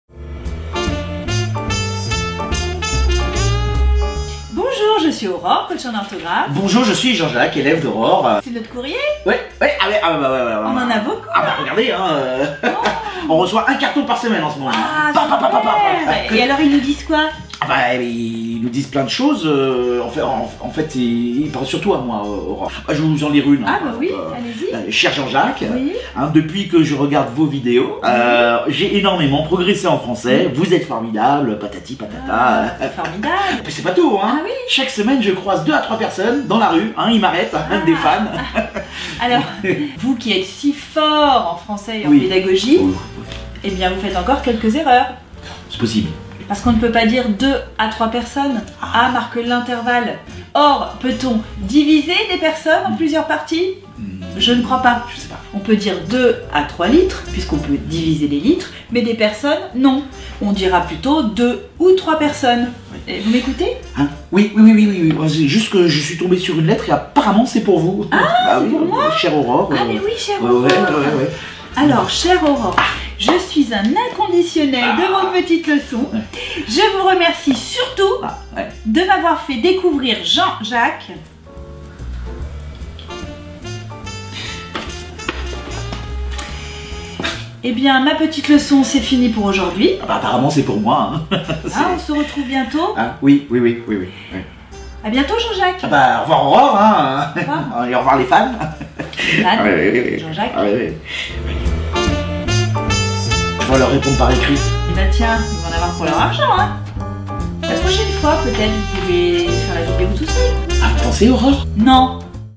Petite leçon de français : "Deux à Trois ?"
Retrouvez tous les 15 jours une petite leçon de français avec Aurore et Jean-Jacques.